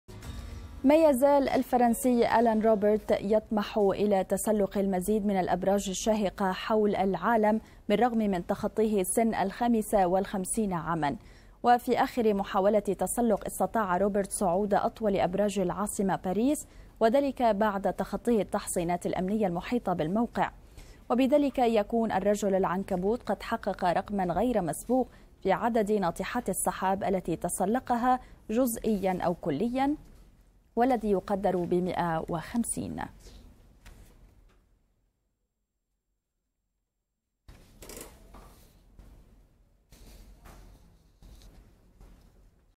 الرجل العنكبوت يتسلق ناطحات السحاب حول العالم
شاهد آيضا:
قيادي بعثي يفضح تاريخ آل الأسد!؟
ما يزال الفرنسي، آلان روبرت، يطمح إلى تسلق المزيد من الأبراج الشاهقة حول العالم، بالرغم من تخطيه سن 55 عاما.
وفي آخر محاولة تسلق، استطاع روبرت صعود أطول أبراج العاصمة باريس، وذلك بعد تخطيه التحصينات الأمنية المحيطة بالموقع. وبذلك يكون "الرجل العنكبوت" قد حقق رقما غير مسبوق في عدد ناطحات السحاب التي تسلقها، جزئيا أو كليا، والذي يقدر بـ 150.
وبدأ الرجل رحلته منذ 11 عاما، واشتهر أكثر بعد تسلقه كلا من برج خليفة في دبي، ودار الأوبرا في سيدني. ومما يثير الاهتمام، السرعة الفائقة التي يتسلق بها "العنكبوت" الأبراج، ففي محاولته الأخيرة، لم يستغرق صعوده ناطحة بارتفاع 200 متر أكثر من ساعة واحدة فقط.